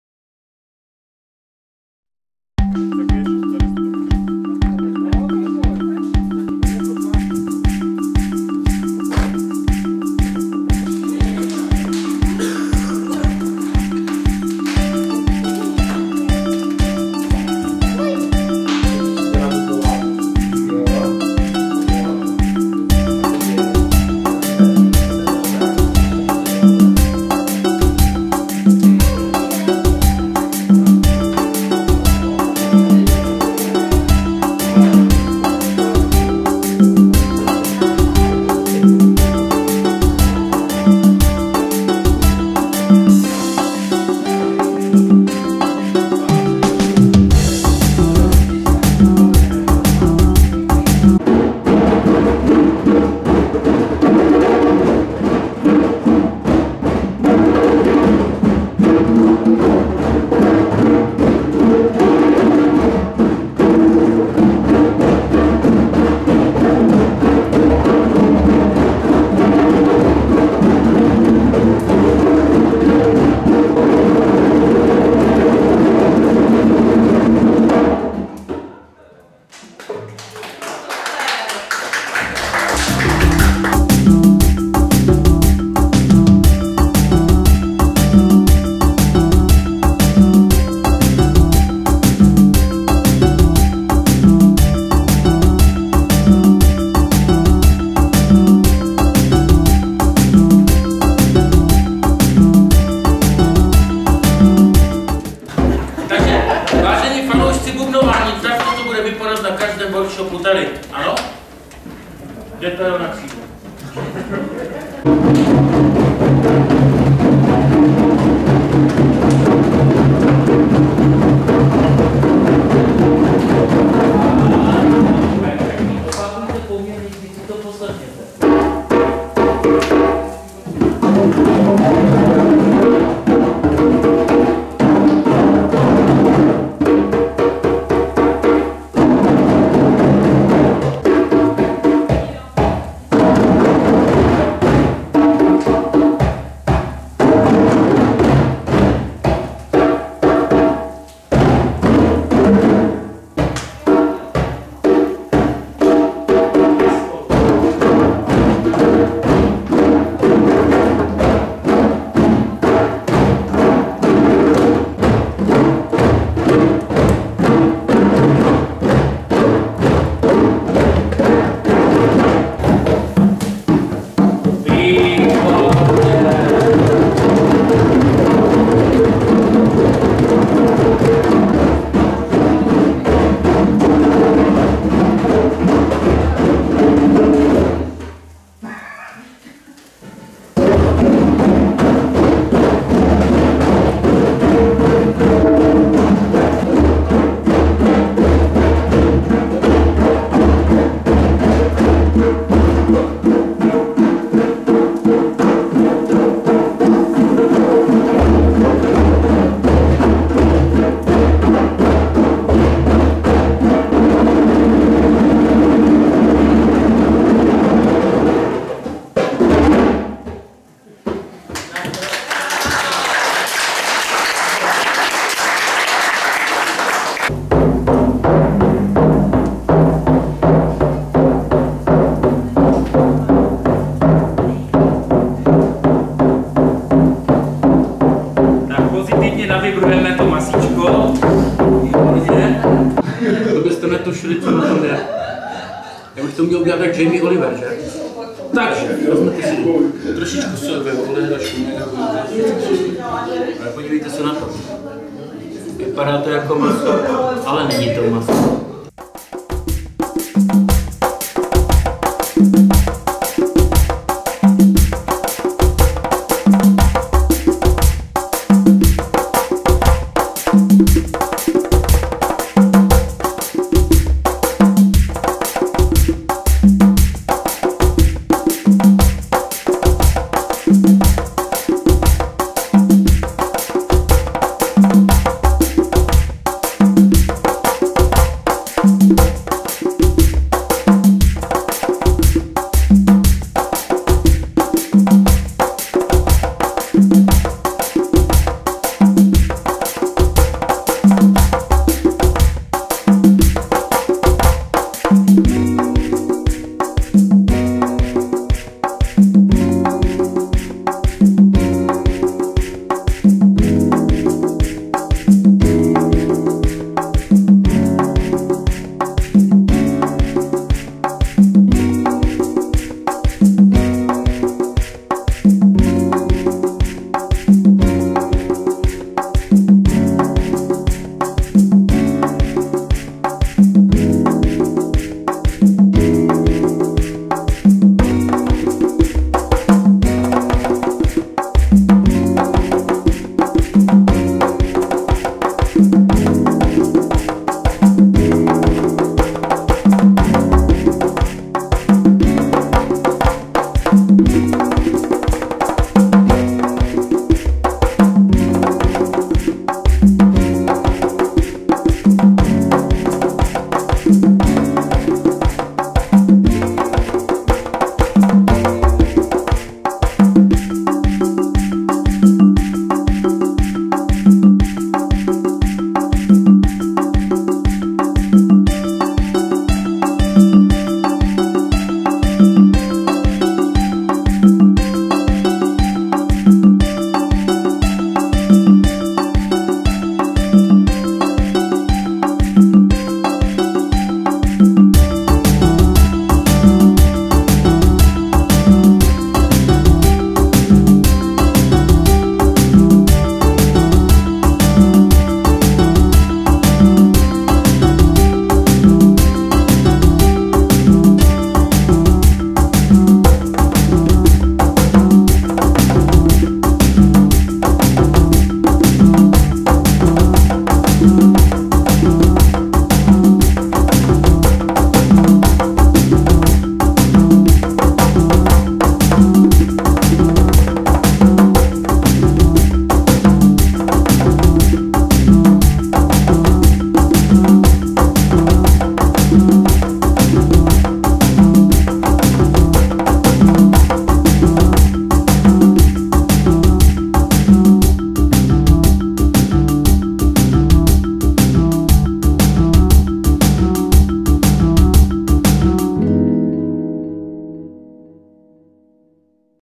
Centrum Slezská Harta - kreativní bubnování s Ivo Batouškem
Bubenický workshop - dílna kreativního bubnování: "Bubnuje celá rodina". V krásném prostředí zrekonstruovaného statku.